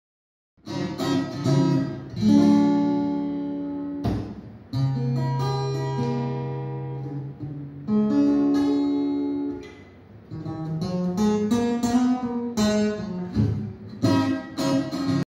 Blazer guitar semi acoustic
Pim plays a very small Blazer guitar at The Fellowship of Acoustics